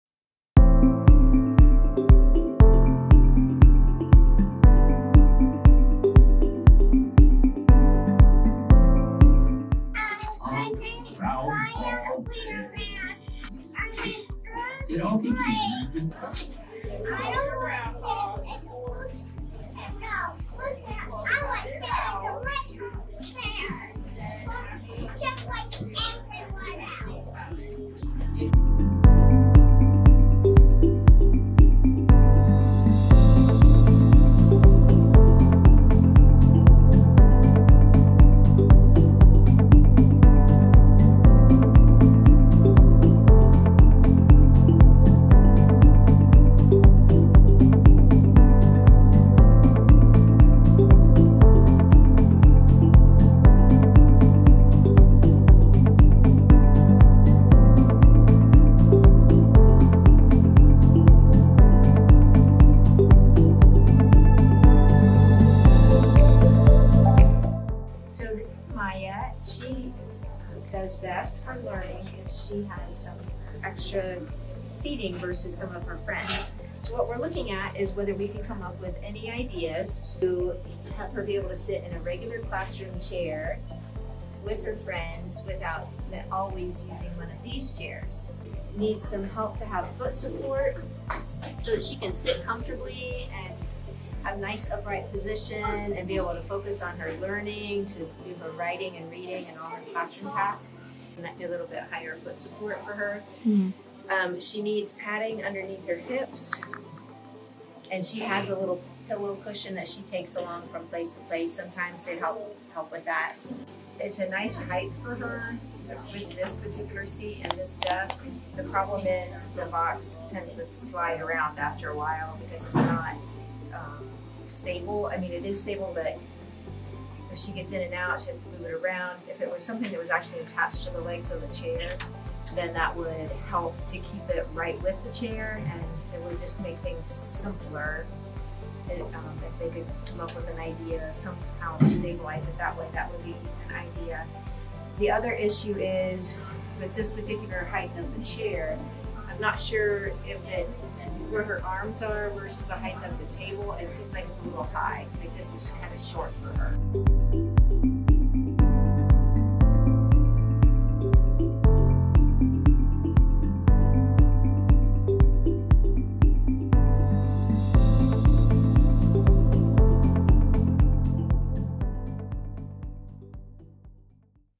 Mayah | Marion Local STEM Invention
Mayah wants to sit in a classroom chair like her friends, but standard chairs aren’t built for her needs. She needs a higher seat, sturdy footrest, padding for her hips, and an adjustable footrest to improve comfort and posture.  These adjustments would help Mayah sit comfortably and engage fully in class, just like her peers.Goodluck!